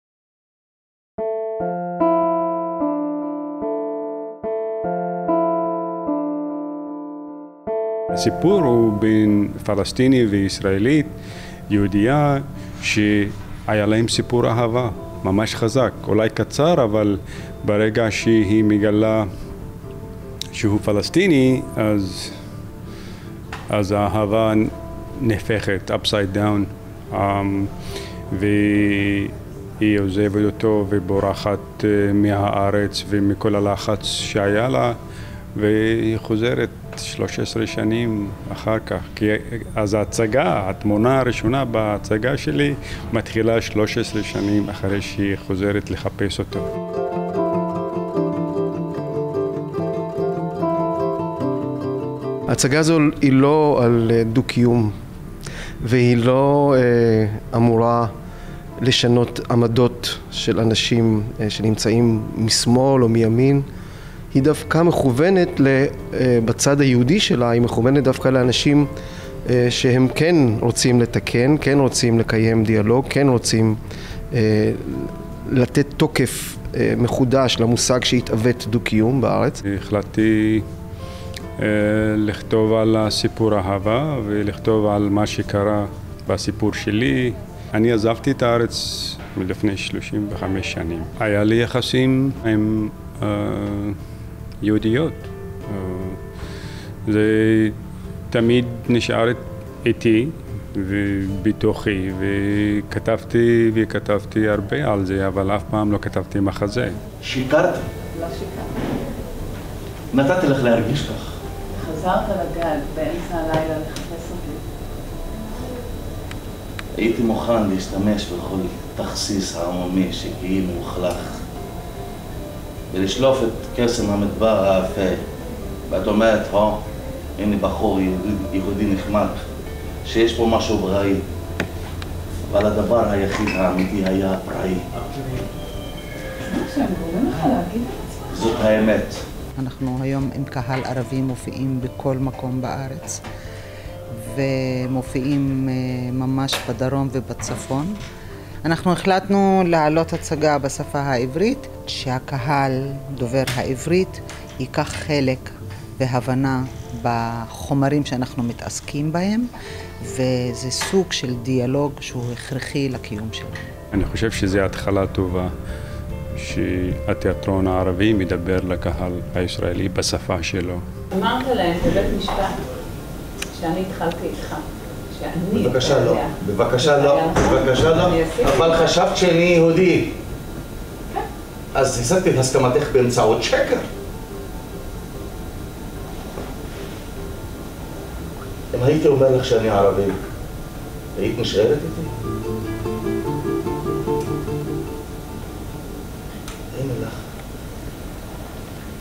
עובד שבת
ההצגה "עובד שבת" הינה ההצגה הראשונה העולה בשפה העברית בתיאטרון הערבי אלמידאן בחיפה. המחזה, שנכתב על ידי חנא עידי ואדוארד מאסט עוסק ביחסי אהבה בין אישה יהודיה וגבר פלסטיני. ההצגה בוימה על ידי סיני פתר ומשחקים בה עינת וייצמן ומחמוד אבו גאזי.